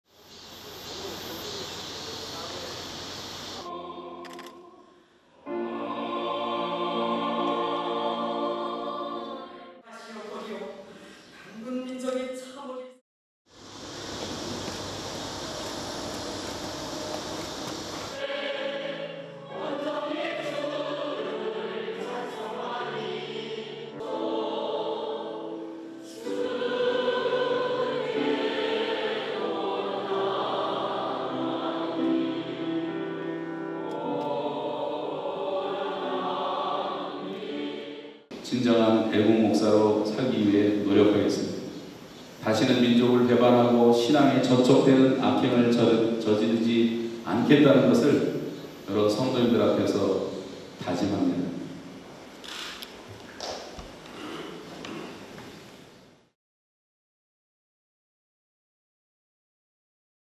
北朝鮮教会で「反省」 拘束のカナダ人牧師
１月に北朝鮮に入国した後、拘束された韓国系カナダ人牧師、ヒョンス・リム氏（６０）が２日、平壌の鳳岫（ポンス）教会で礼拝に参加した。体制転覆を図ったとして拘束されたリム氏は、参加者らの前で「懺悔するためこの場に立った」と“反省”の弁を述べた。